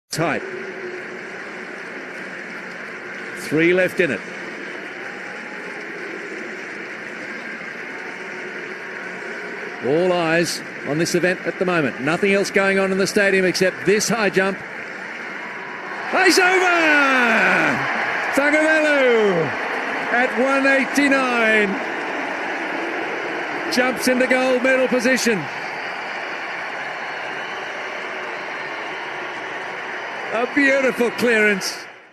Mariyappan Thangavelu's gold medal jump in Men's T42 High Jump final - Rio 2016 Paralympics
Mariyappan Thangavelu's gold medal jump in Men's T42 High Jump final - Rio Paralympics